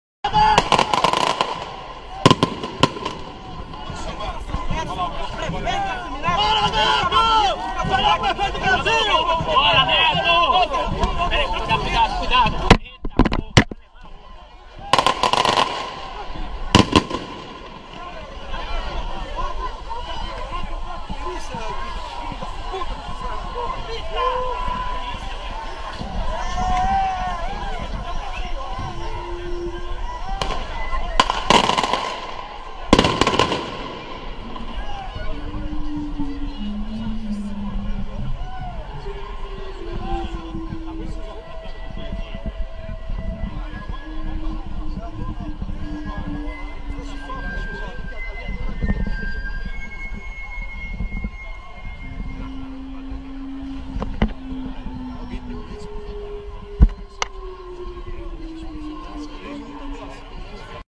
Manifestantes jogam ovo em João Doria, em Salvador-BA
Doria foi à cidade para receber o título de cidadão soteropolitano.